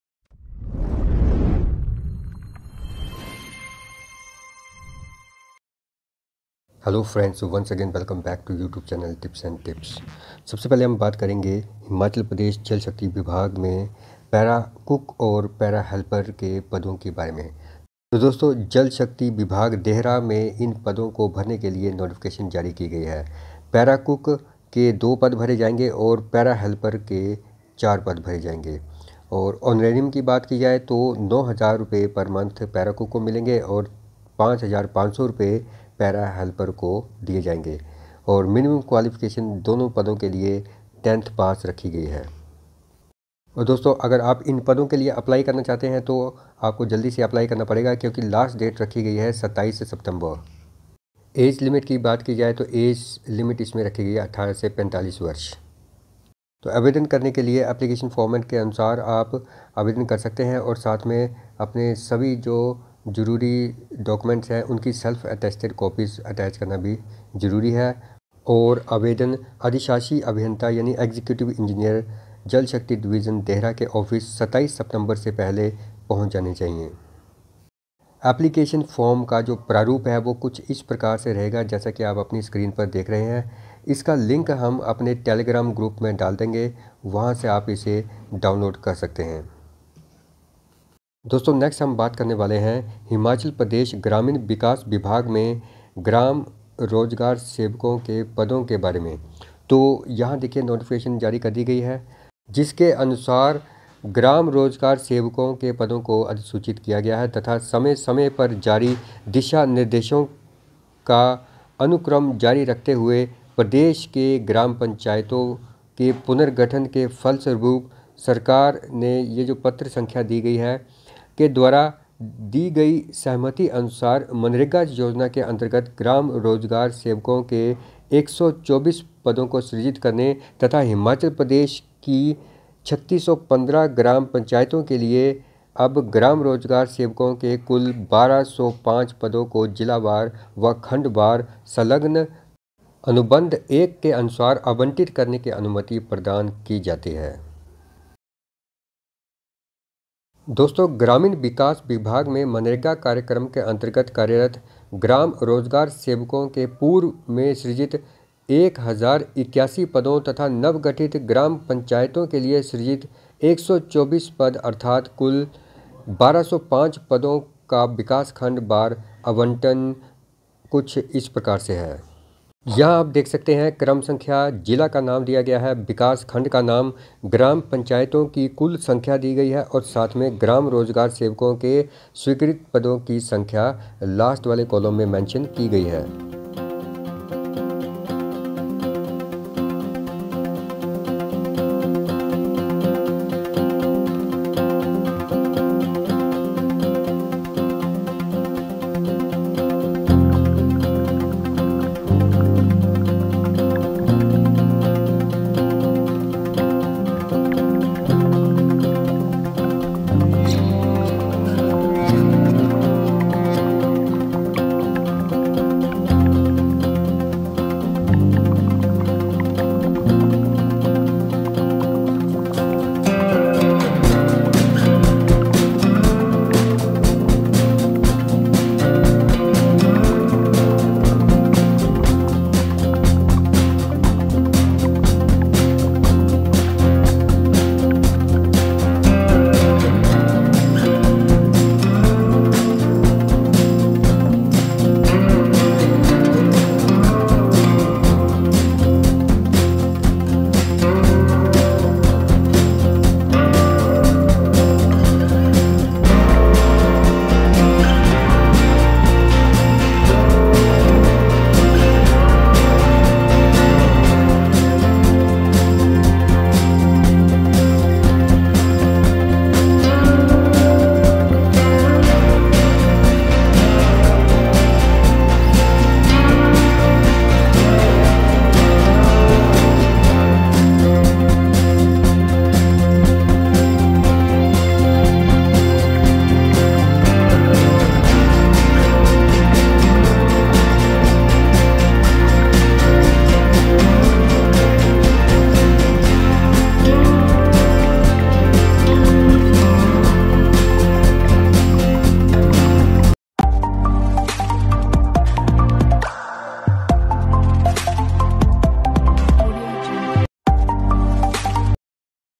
Para Cook and Para Helper Bharti | HP Jal shakti vibhag | ग्राम सेवकों के 1205 पद
HP Gramin vikas vibhag mein 1205 pad srijit honge
हिमाचल ग्रामीण विकास विभाग में ग्राम सेवकों के 1205 पद स्वीकृत